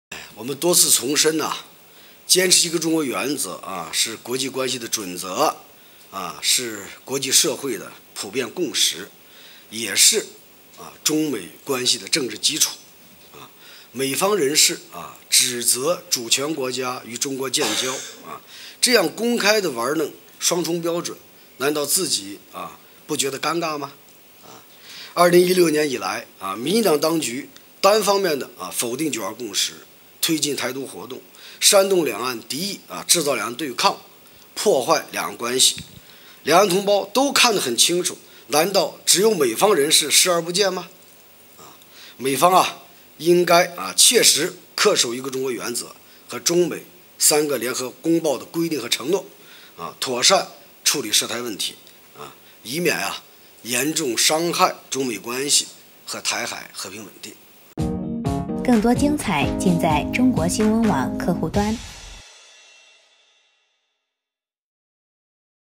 美方指责主权国家与中国建交 国台办：“双重标准”不尴尬吗？
9月25日，国务院台湾事务办公室举办新闻发布会。国台办发言人马晓光谈“美官员支持台所谓的‘国际空间’”表示，坚持一个中国原则是国际关系准则，是国际社会普遍共识，也是中美关系的政治基础。美方人士指责主权国家与中国建交，这样公开地玩弄“双重标准”，难道自己不觉得尴尬吗？